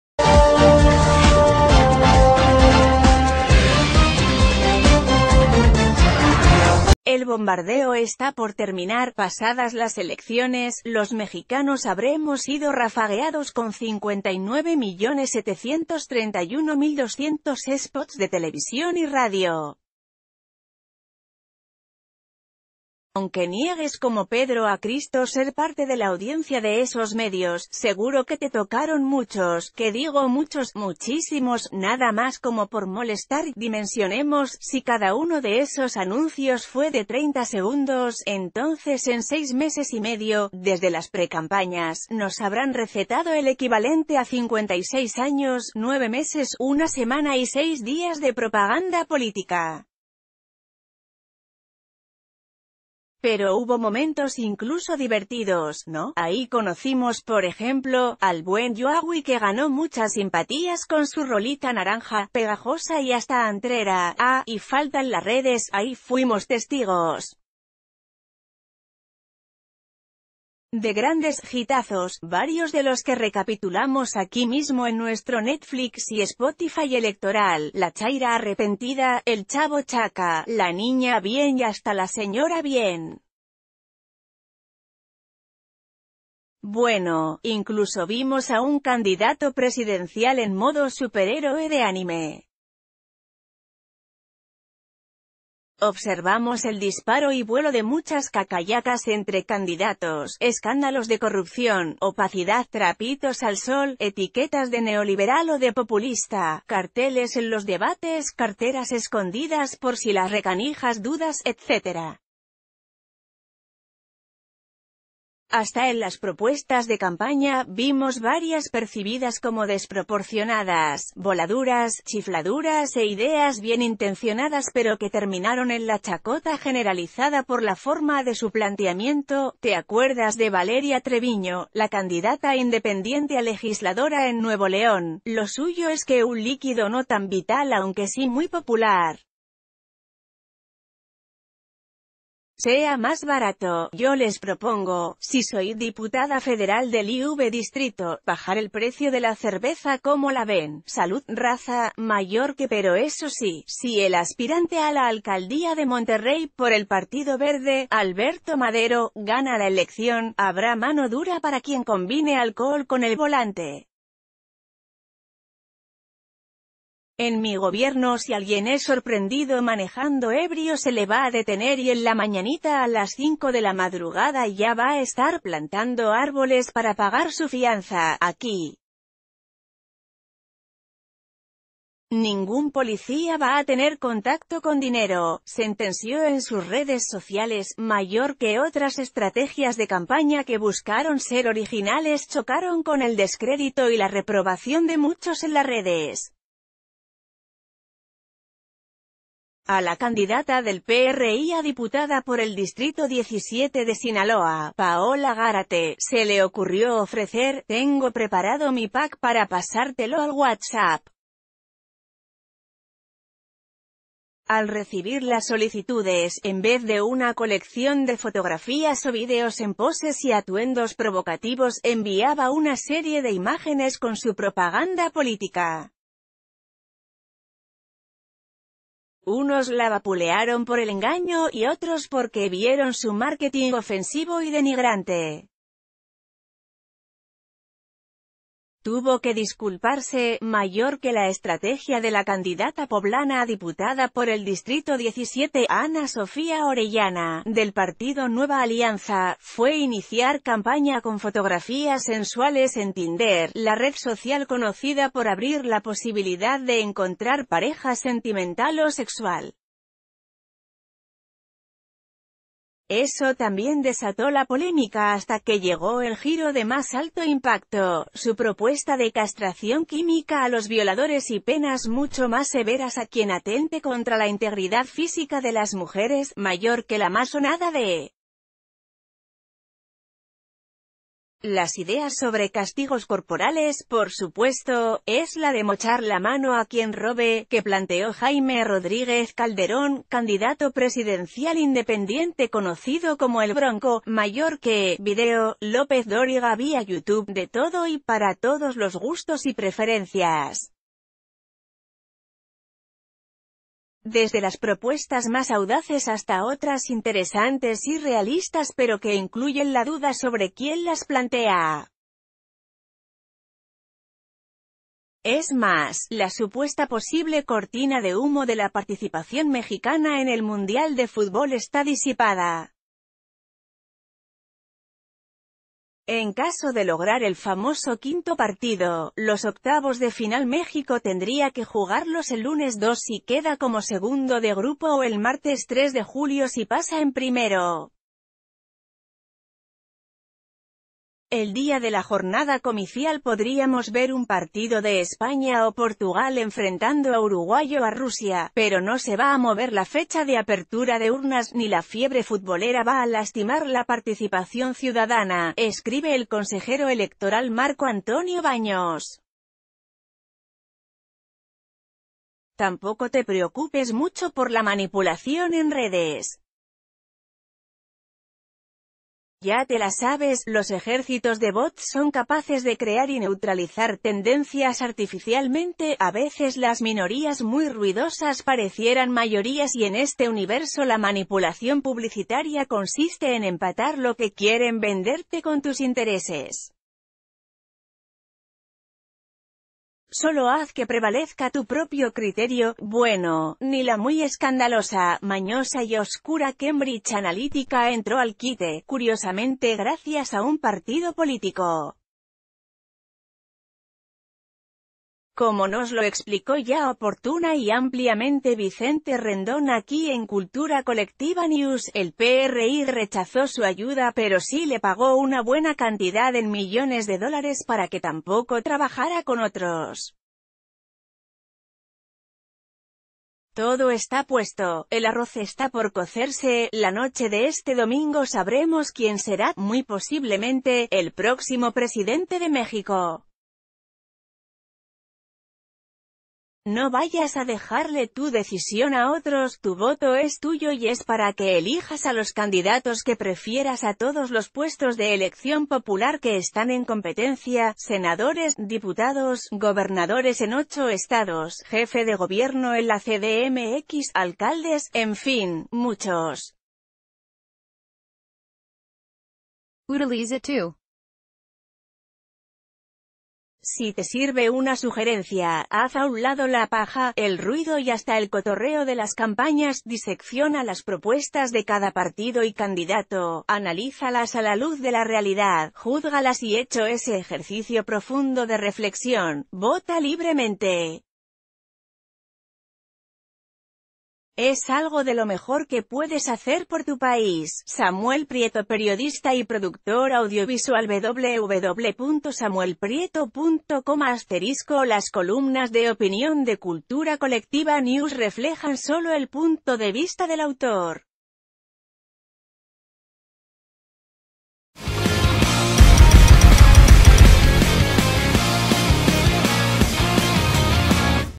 OPINIÓN: ConcienciaParaElegir
OPINIÓN

El bombardeo está por terminar. Pasadas las elecciones, los mexicanos habremos sido rafagueados con 59 millones 731 mil 200 spots de televisión y radio. Aunque niegues como Pedro a Cristo ser parte de la audiencia de esos medios, seguro que te tocaron muchos, qué digo muchos, muchísimos. Nada más co...